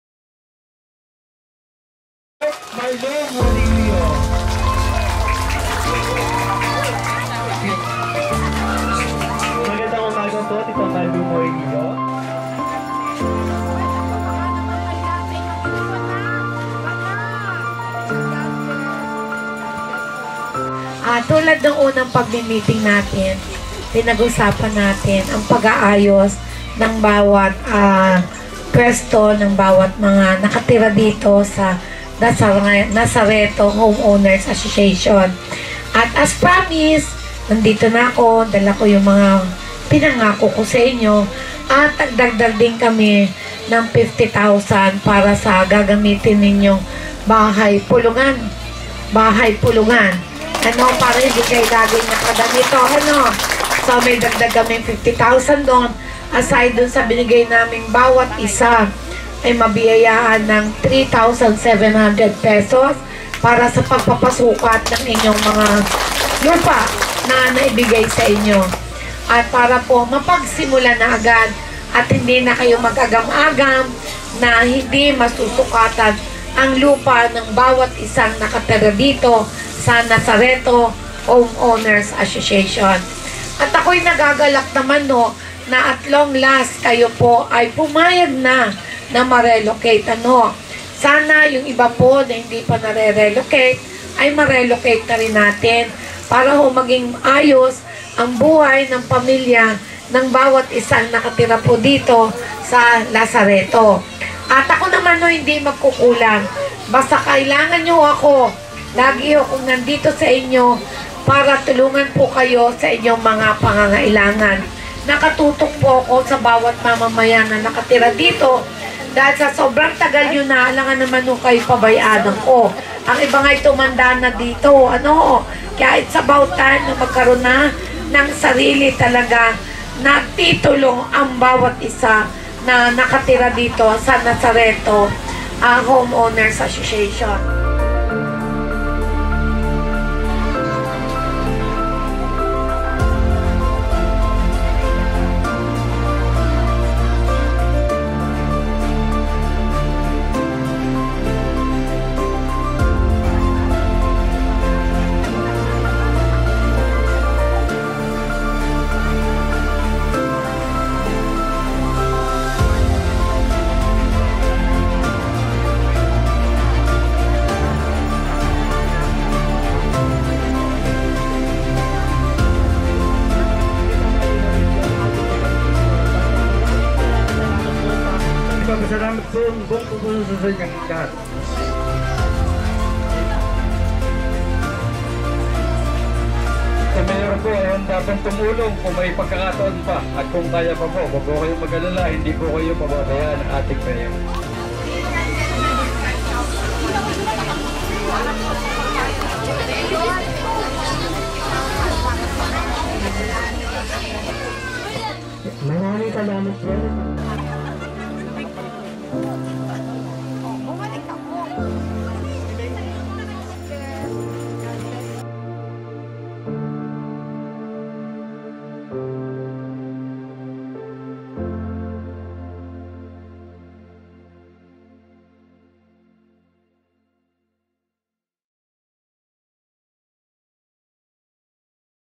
Nazareto Homeowners Association | July 9, 2024
ICYMI | Ipinagkaloob ng 𝗖𝗶𝘁𝘆 𝗚𝗼𝘃𝗲𝗿𝗻𝗺𝗲𝗻𝘁 𝗼𝗳 𝗖𝗮𝗹𝗮𝗽𝗮𝗻 sa 𝗡𝗮𝘇𝗮𝗿𝗲𝘁𝗼 𝗛𝗼𝗺𝗲𝗼𝘄𝗻𝗲𝗿𝘀 𝗔𝘀𝘀𝗼𝗰𝗶𝗮𝘁𝗶𝗼𝗻 ang tulong pinansyal na ipinangako ni 𝗖𝗶𝘁𝘆 𝗠𝗮𝘆𝗼𝗿 𝗠𝗮𝗿𝗶𝗹𝗼𝘂 𝗙𝗹𝗼𝗿𝗲𝘀-𝗠𝗼𝗿𝗶𝗹𝗹𝗼 na nagkakahalaga ng 𝗣𝟴𝟮𝟱,𝟭𝟬𝟬.𝟬𝟬, bilang pambayad sa individual lot survey ng nasa 𝟯𝟰𝟯 na miyembro at tulong pinansyal sa pagpapagawa ng kanilang Bahay-pulungan. 💜🏡